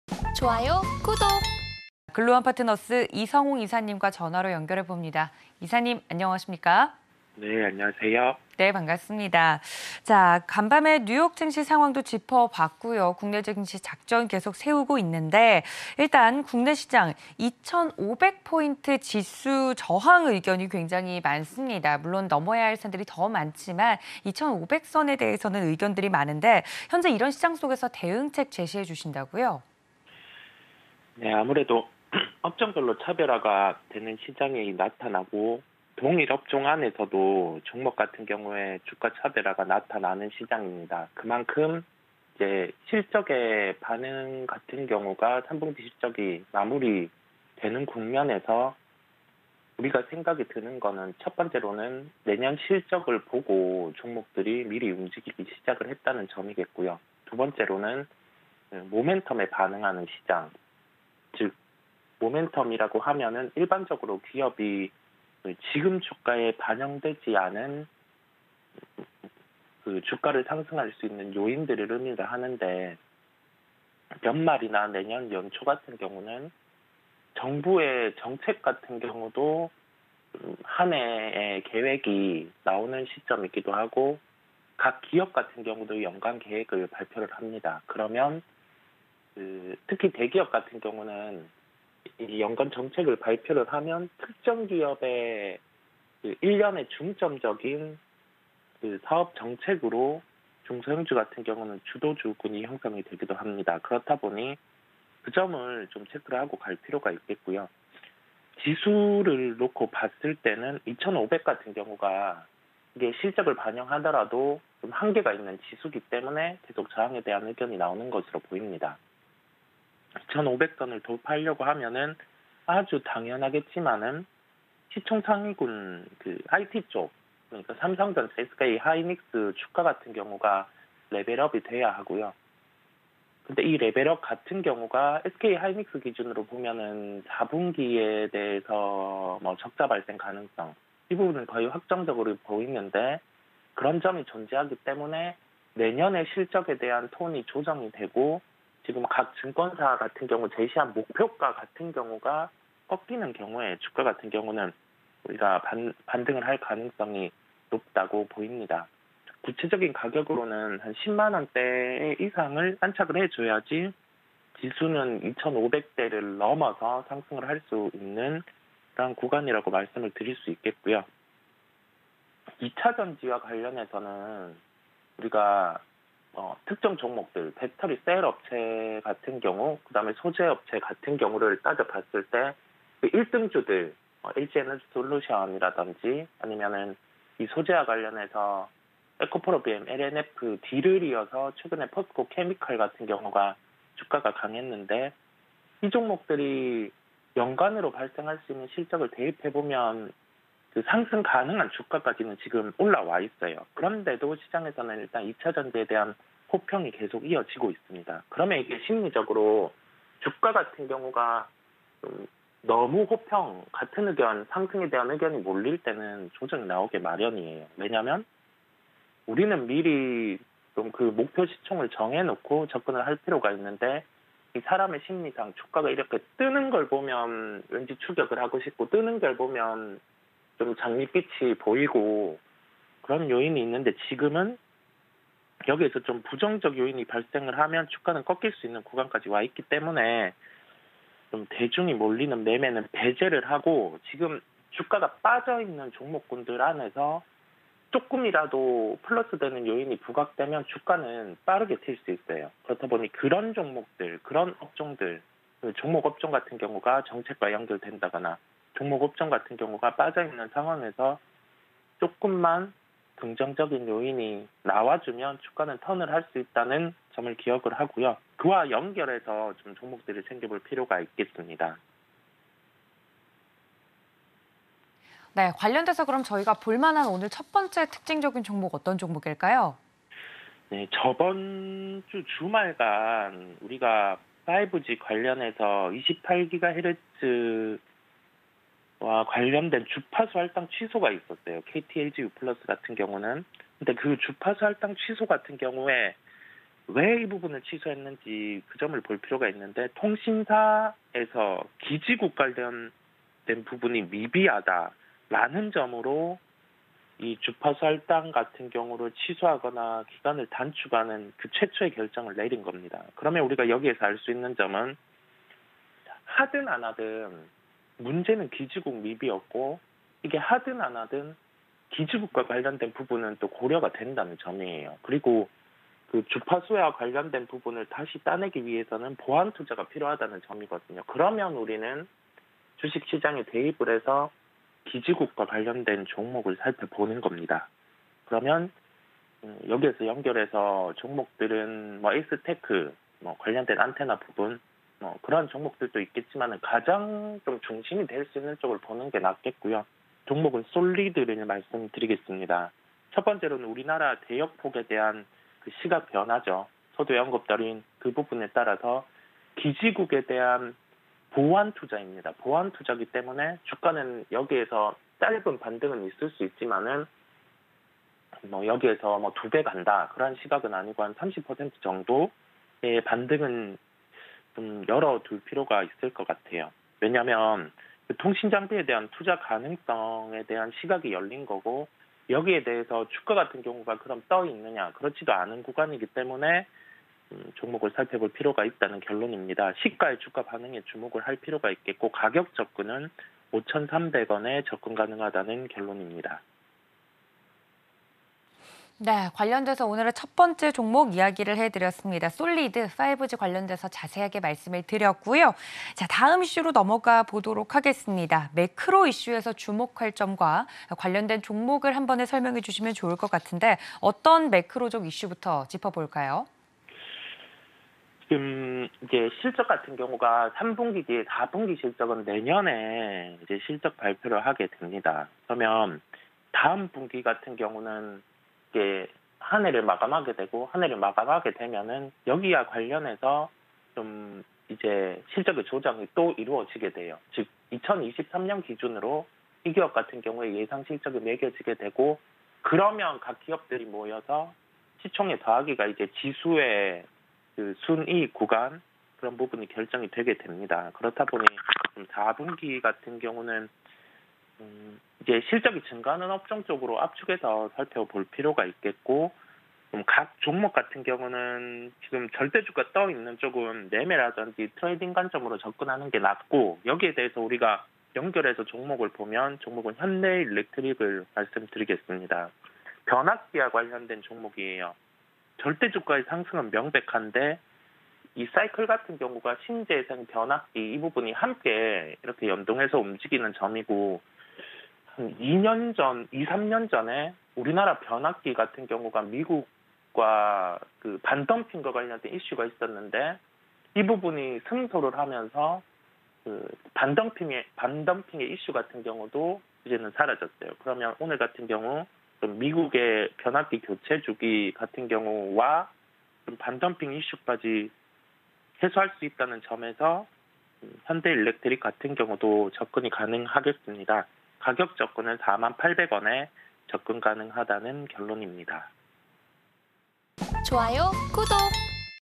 코스피 2500 경계감, 대응책은? │
시총 상위군 IT 레벨업 여부
2차전지군에 대한 호평이 많은 것은 고민
대중이 몰리는 매매는 배제 필요

이성웅 이사의 특징주 – 쏠리드
우리나라 대역폭에 대한 시각 변화
통신장비에 대한 투자 가능성
시가 주가 반응에 주목
5,300원 접근 가능

매크로 이슈에서 주목할 점은?
다음 분기 실적 집중 업종 체크 필요
매매 트레이딩 관점

이성웅 이사의 특징주 – 현대일렉트릭
절대주가의 상승은 명백
싸이클에 대한 관심
다음 분기까지 실적 연속성
40,800원 접근 가능

이 채널에서 더 많은 정보 받으실 수 있습니다~!